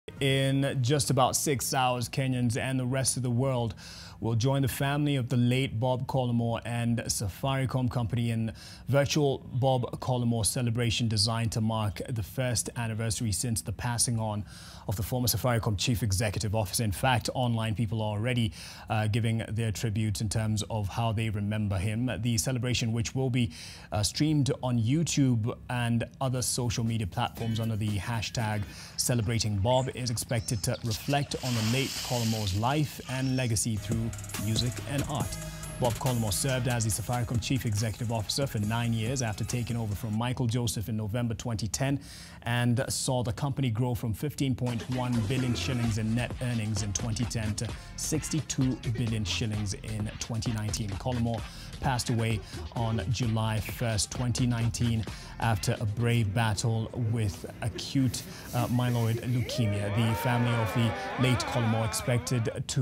CelebratingBob: Former Safaricom CEO Bob Collymore’s first anniversary
In the next six hours, Kenyans and the rest of the world will join the family of the late Bob Collymore and Safaricom Plc in the virtual Bob Collymore Celebration designed to mark the first anniversary since the passing on